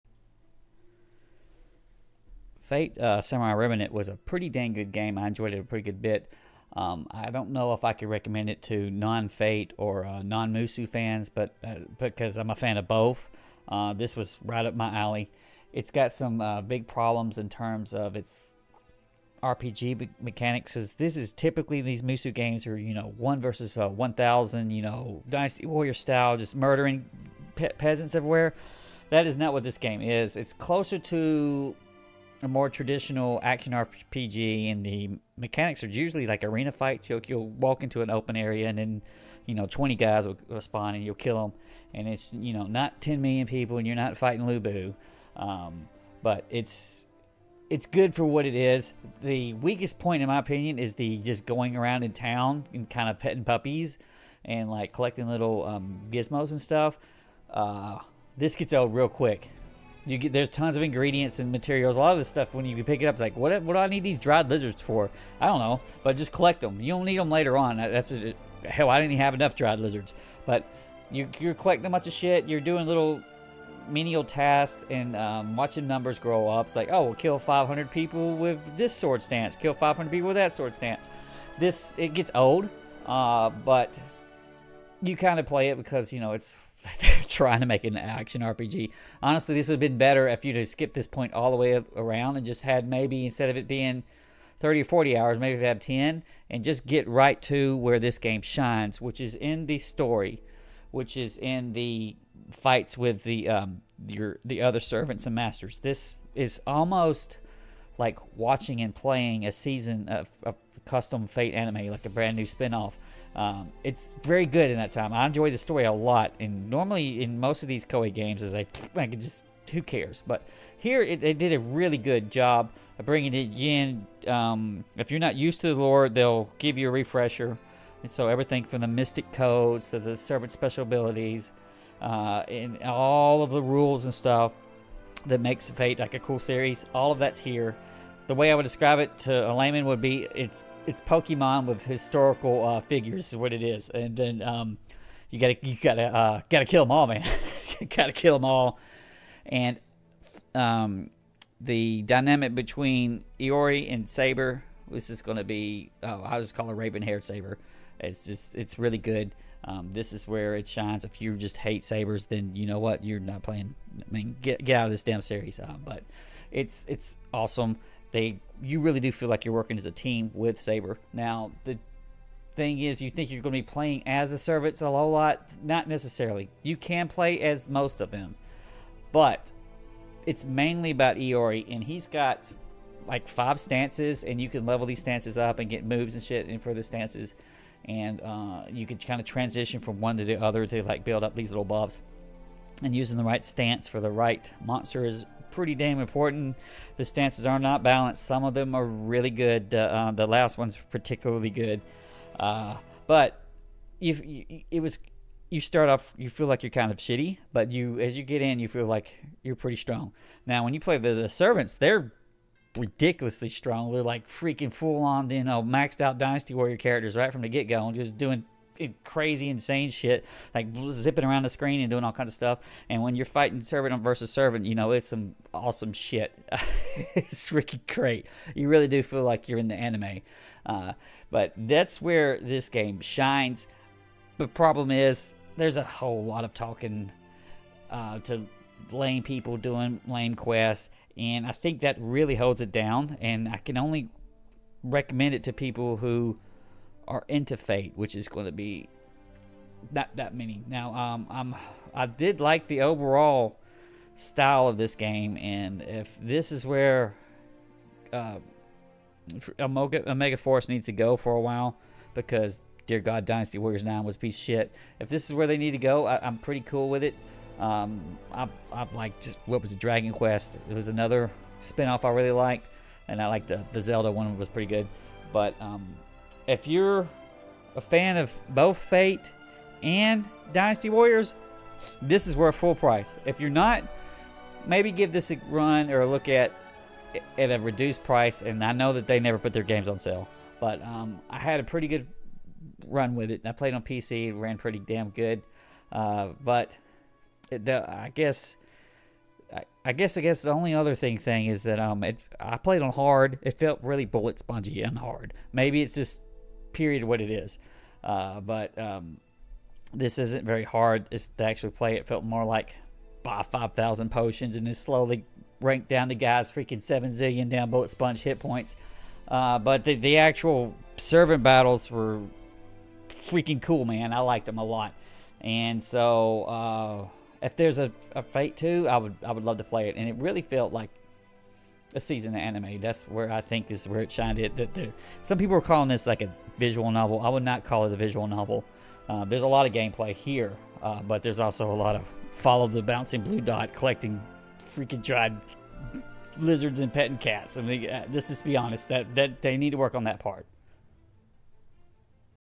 Fate/Samurai: Remnant Quick Review | Fate fans should check it out!
Koei has a long history of hits and blunders when it comes to musou games. They definitely care with Fate/Samurai and it feels like watching a spin off anime series. I can recommend this to Fate fans provided they also like anime brawlers. This is much closer to an action RPG than the typical one vs. a thousand musou and while the Servant fights are great, everything else is kind of lacking and samey.  In particular, the town segments are a drag of collecting dried lizards and petting dogs and cats and murdering peons in squared off areas.

Positives
- Story is great. Sabre is very likeable.
- Servant fights are intense
- Very faithful to the series.
- new game plus with VN style different routes (I only beat it the first time)

Negatives
- Areas look way too similar. Gets old quick.
- Town Exploration is weak and boring
- You do not get to control servants enough
- Most enemies are so slow and armored forcing a passive counter style fight